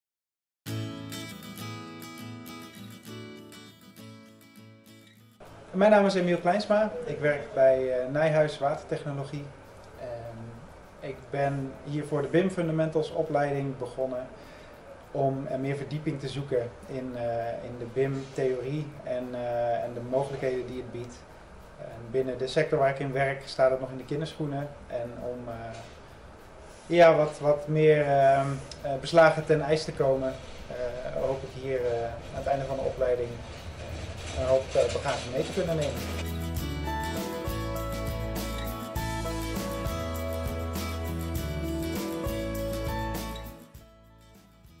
Waarom de BIM fundamentals opleiding volgen? | Avans+
Emiel zoekt meer verdieping in de BIM materie. Daarom volgt hij de BIM fundamentals, zodat hij bij zijn organisatie BIM naar een hoger niveau kan krijgen.

Ga naar en download de brochure voor jouw opleiding!